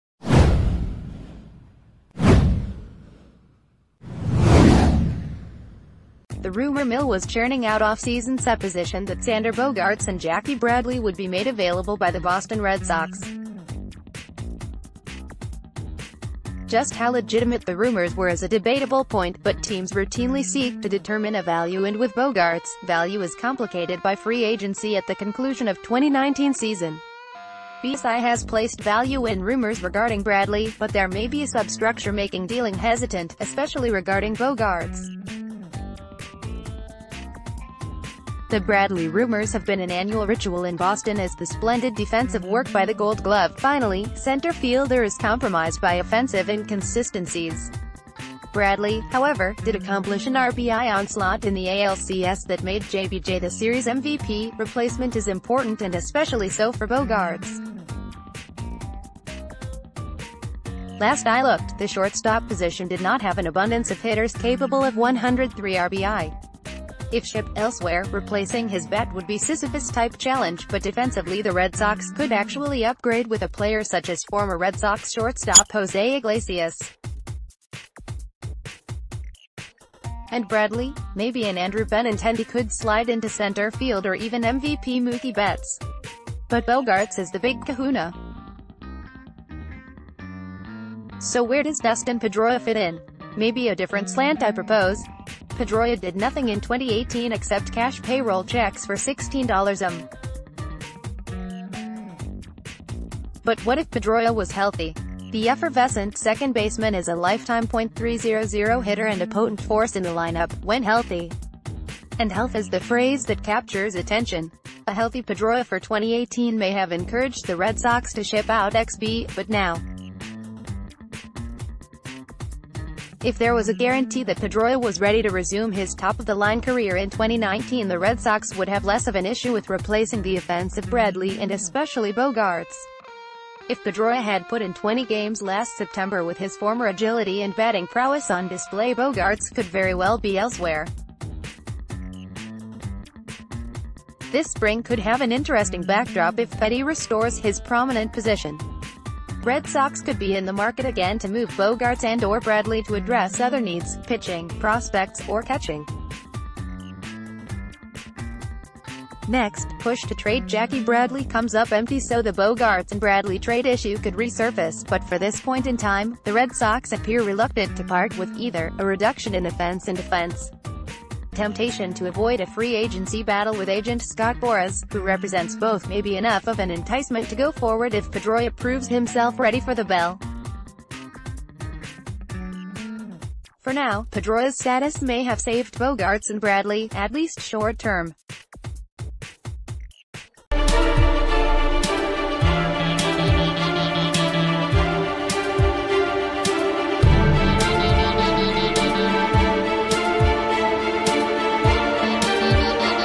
Red Sox: Dustin Pedroia may have saved Xander Bogaerts, Jackie Bradley
The rumor mill was churning out offseason supposition that Xander Bogaerts and Jackie Bradley would be made available by the Boston Red Sox. Just how legitimate the rumors were is a debatable point, but teams routinely seek to determine a value and with Bogaerts, value is complicated by free agency at the conclusion of 2019 season. BSI has placed value in rumors regarding Bradley, but there may be a substructure making dealing hesitant – especially regarding Bogaerts. The Bradley rumors have bee...